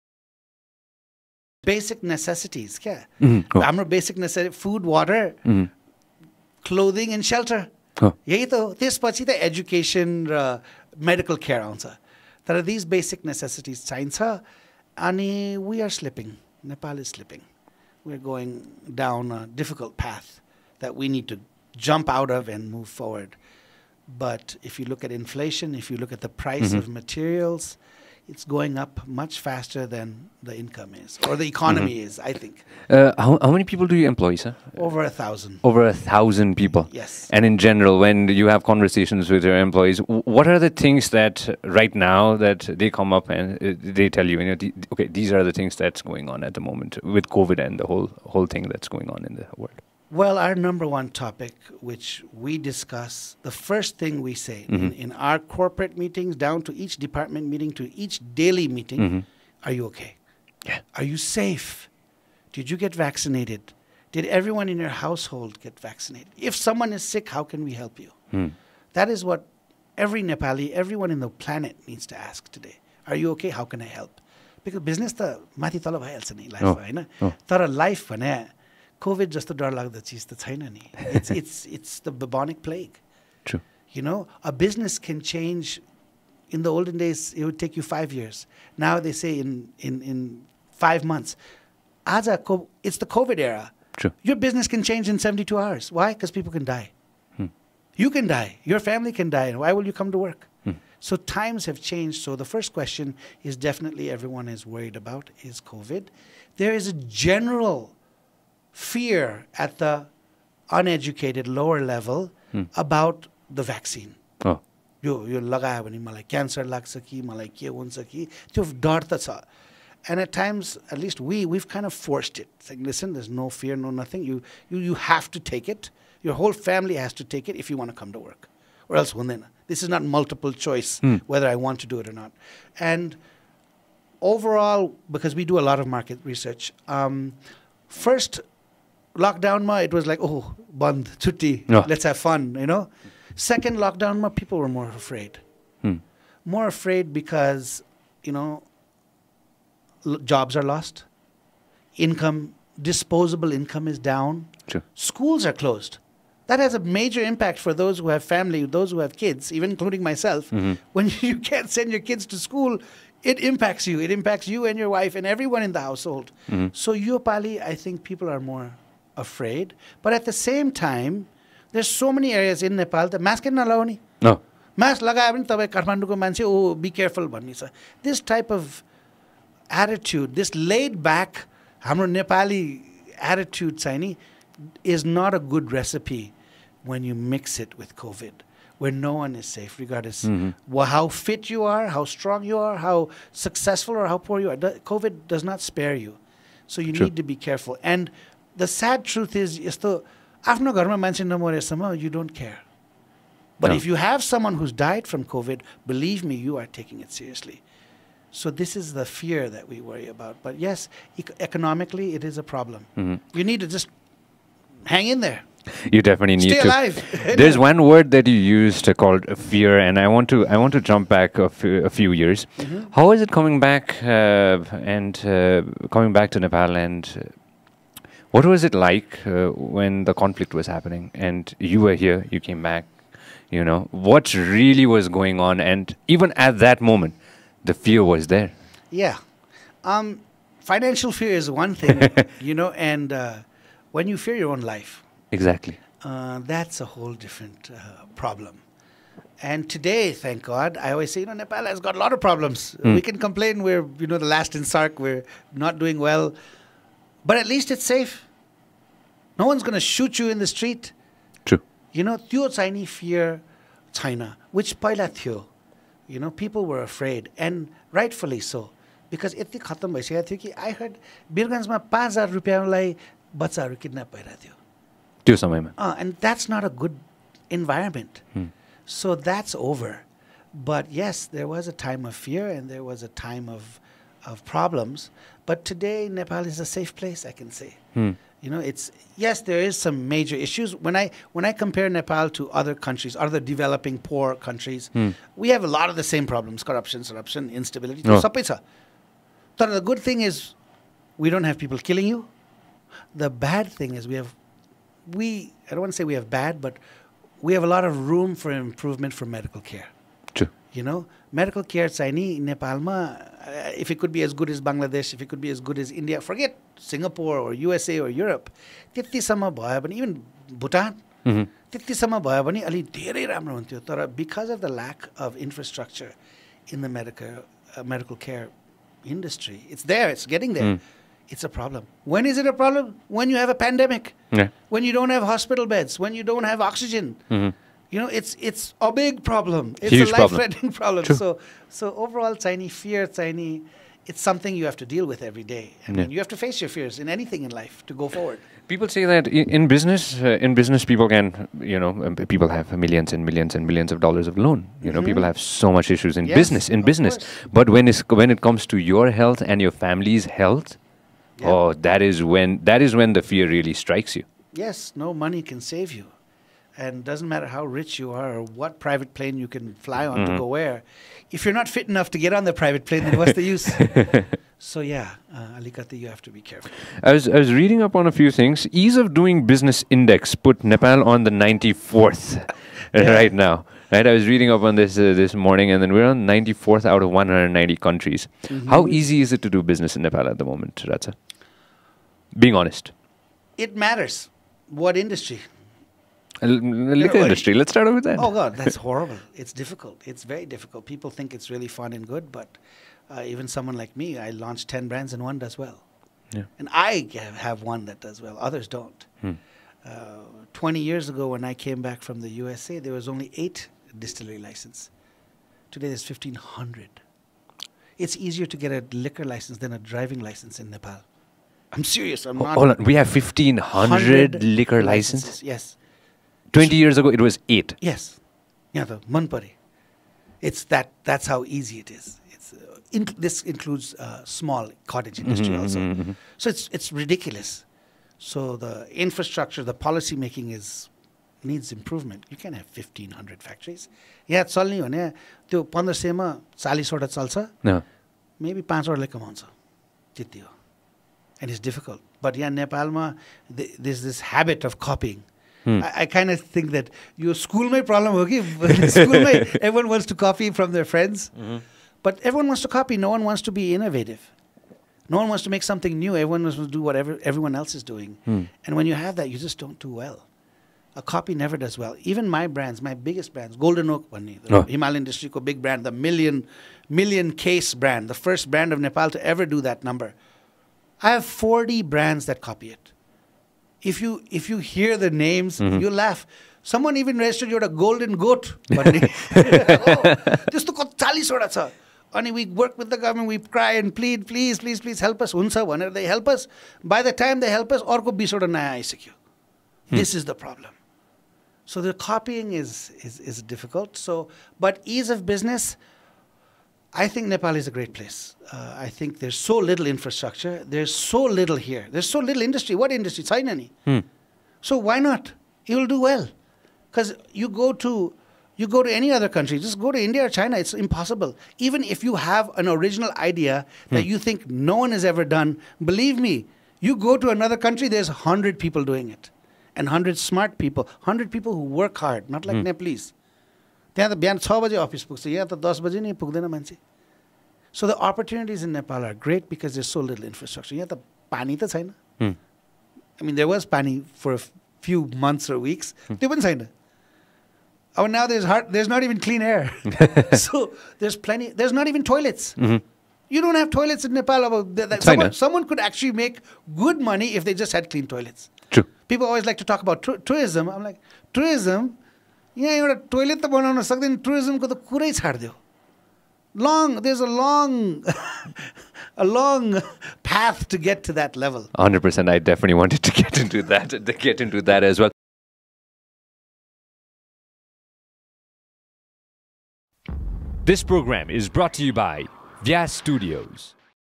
Raj Bahadur Shah on Copycat Business
Watch Raj Bahadur Shah, the Managing Director of Jawalakhel Group of Industries, share his perspectives on various issues relevant to us! You don’t want to miss this.

Thank you for being with us!